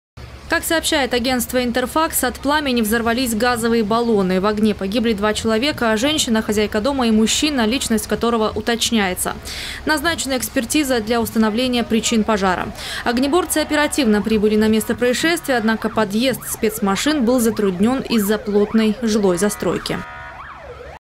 В Адлере полностью выгорел двухэтажный дом. Новости Сочи Эфкате
ЧП произошло на Белорусской, 20 ночью 11 декабря.
Сочи. Новости.
Телеканал Эфкате.